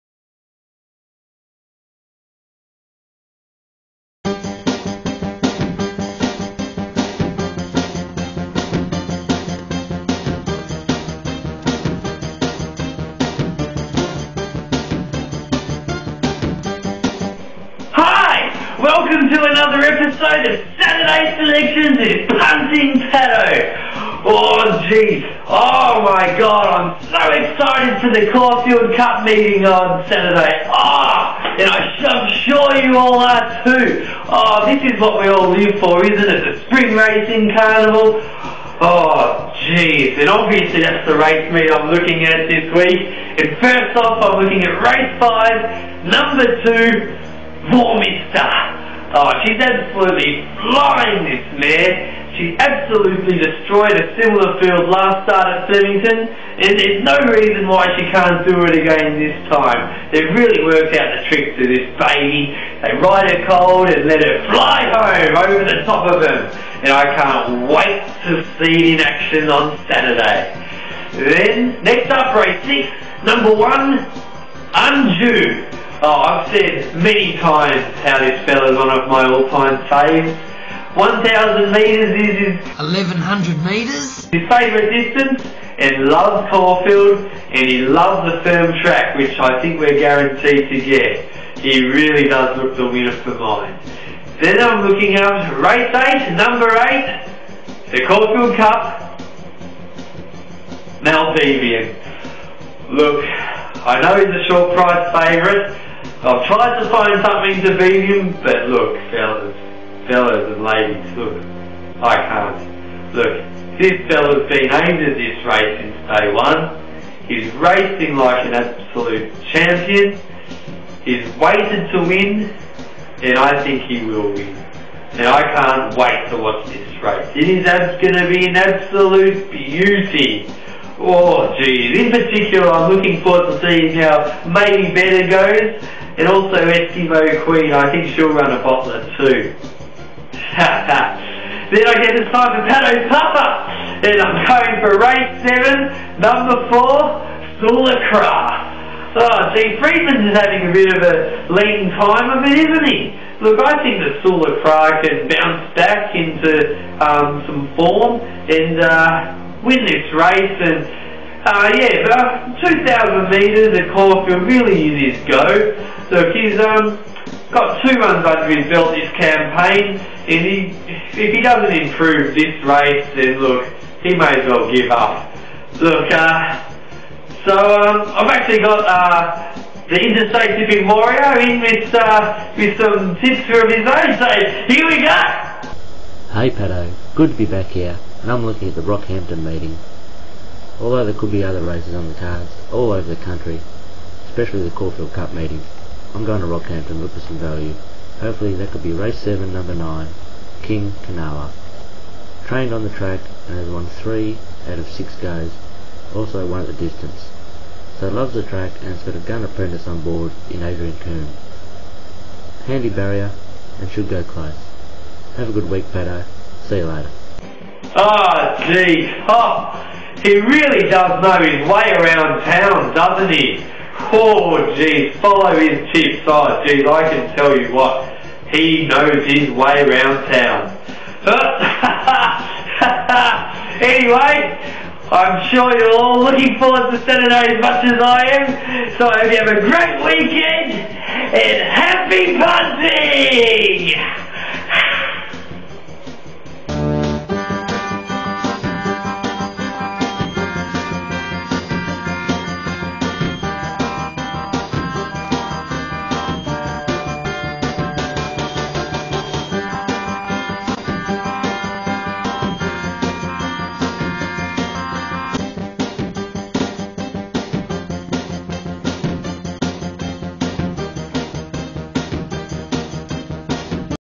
Caulfield 20th October 2007
Punting Patto fills us in on his 3 best bets for the horse races on Caulfield Cup day!  Also features 'Pattos Puffer' as well as the 'Interstate Tipping Warrior'!!!